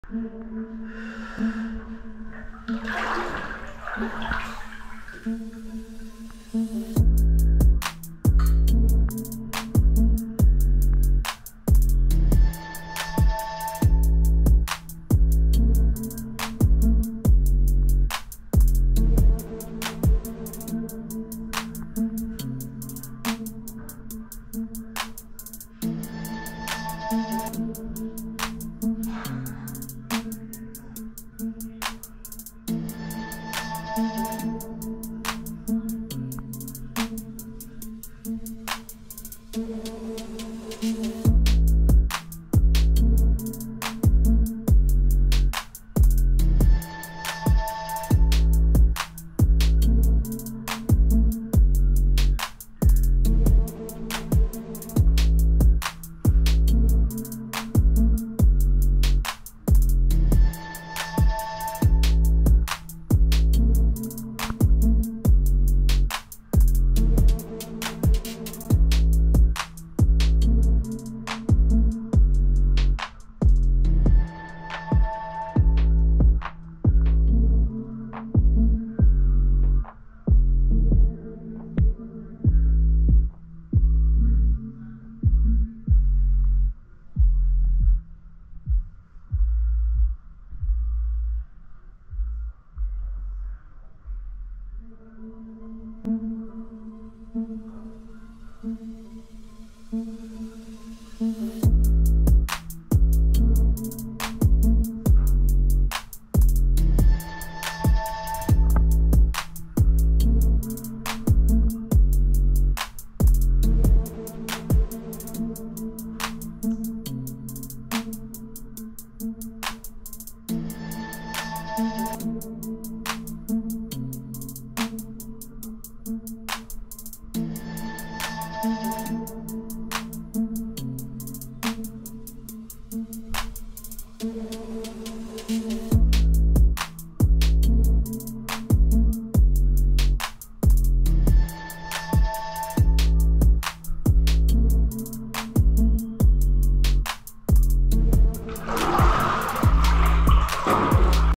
bathtub breath hold challenge (2 min 24 seconds)
Hey guy's in today's video I will do the
* faceup,
* nose unplugged,
* eyes open
* mouth open
 bathtub challenge let's go!